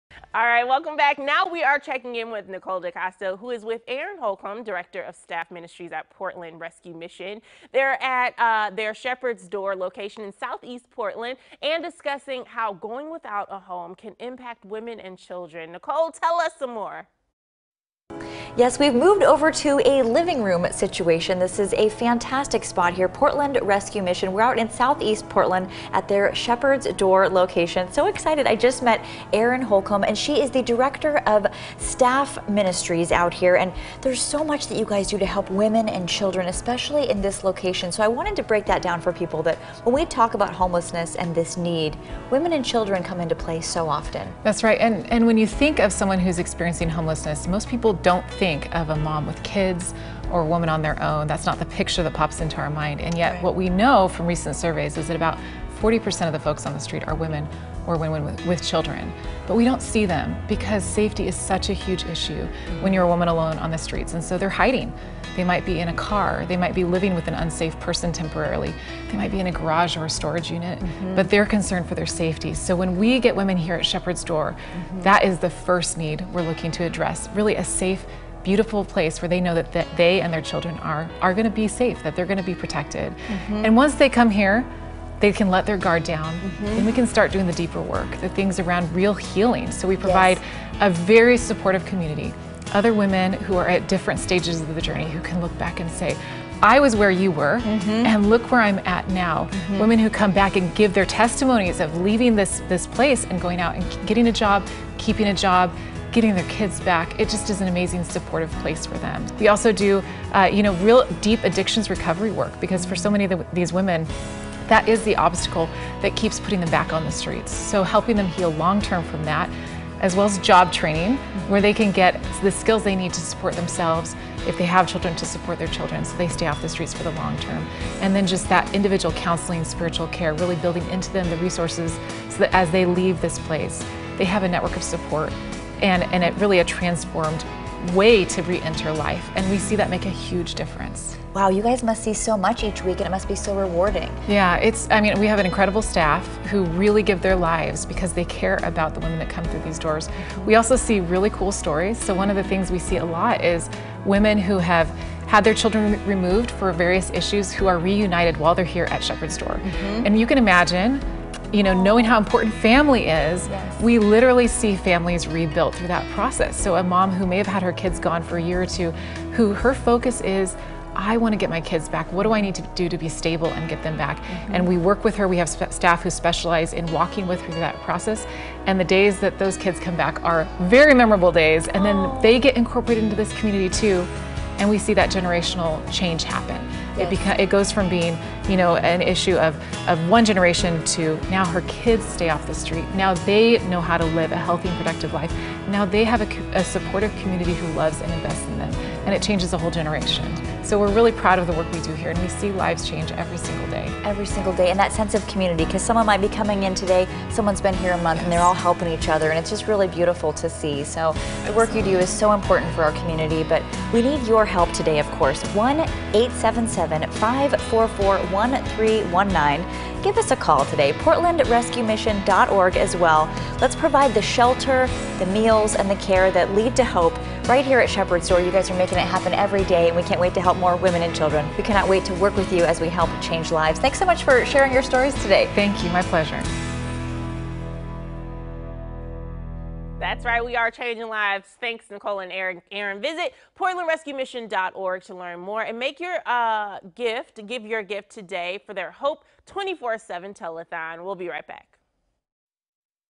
Learn How Portland Rescue Mission Helps Women and Children
Today we're giving the gift of hope. We're helping Portland Rescue Mission for their 24/7 Telethon today (Sept. 29, 2022). And you can give at PortlandRescueMission.org.

More information on Nicole DeCosta:
Web: NicoleDeCosta.com
For: Portland's CW and KOIN 6 (KOIN.com)